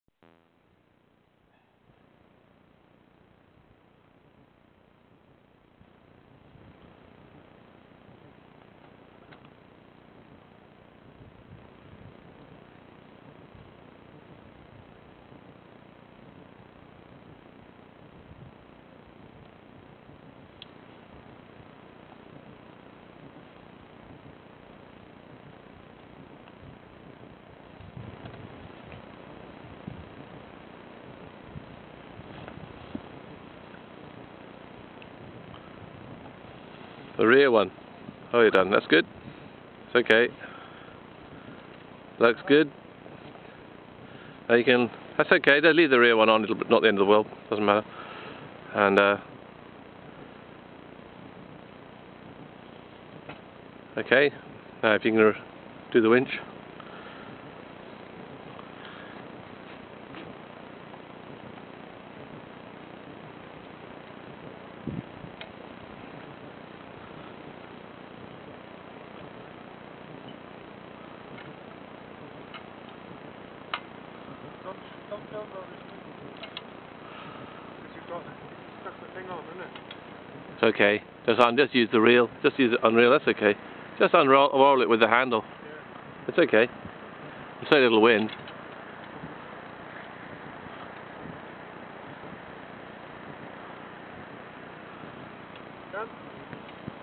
Helikite Aerostat for Lifting High Altitude Military Radio-Relay Helikite .AVI
This shows the rapid deployment of a Skyhook Helikite lifting a radio-relay payload. This Helikite went to 1000ft providing excellent comms for the trial. Over the week trial it flew perfectly in all weathers including: High winds, nil-wind, rain, sleet, fog and sub-zero temperatures. The competing UAV crashed repeatedly until it exited the trial and the mast froze in the down position. Launch at Imber, Salisbury Plain, UK. Dec 2009